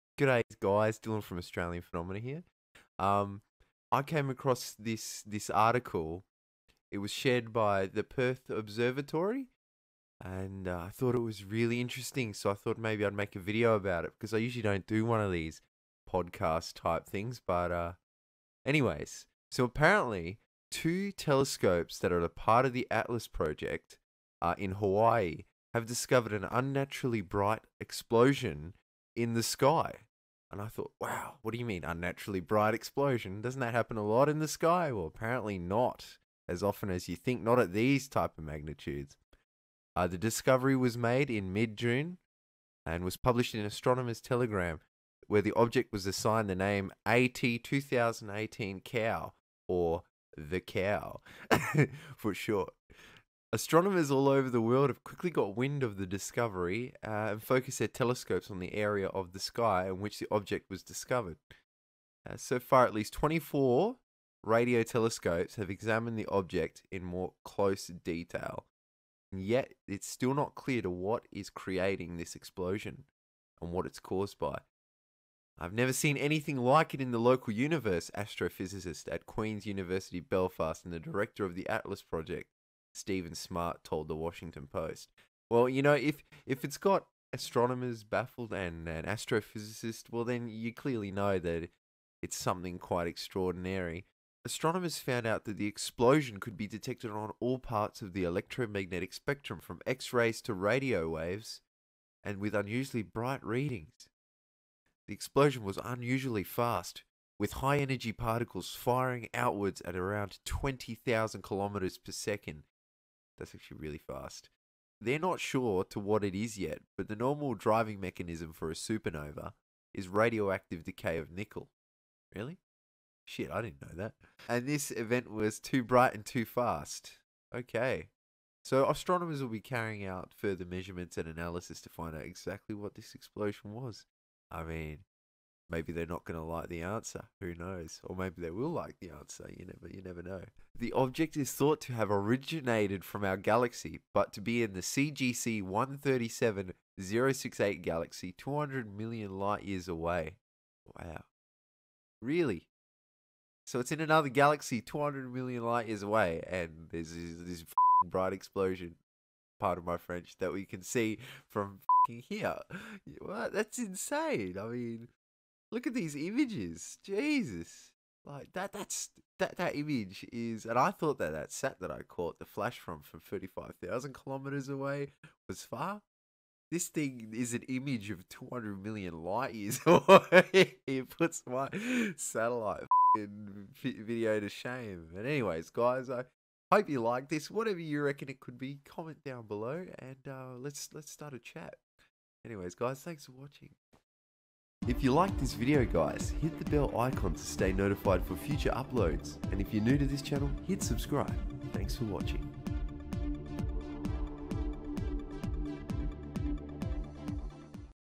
Mysterious Space Object Is Baffling Astronomers!
Object 10x brighter than a normal supernova!